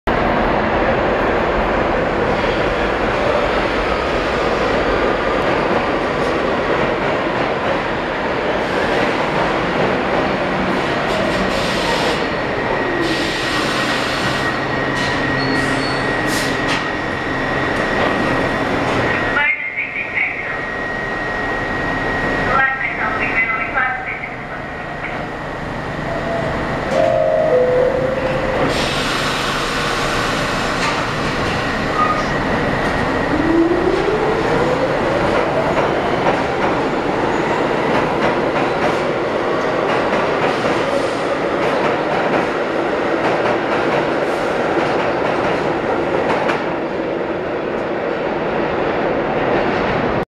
3 train at Bergen Street
New Lots Avenue-bound 3 train of R62s arriving at, stopping and leaving Bergen Street